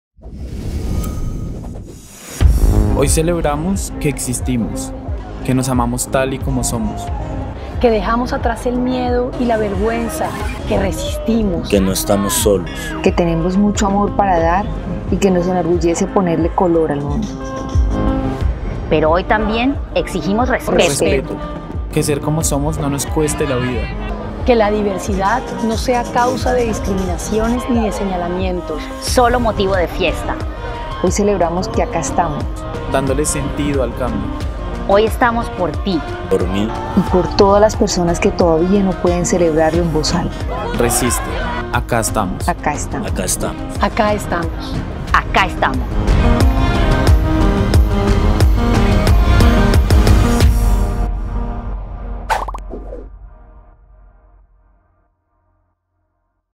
Marcha del orgullo LGBT Bogotá 2021 | Pride
Poder decir con orgullo "acá estoy y me acepto como soy", es un camino que muchas está lleno de obstáculos. El Pride es un día para celebrar la libertad. Sentiido también dice con orgullo: "Existimos, acá estamos".
Video por: Go Team
Imágenes de la Marcha LGBT de Bogotá 2021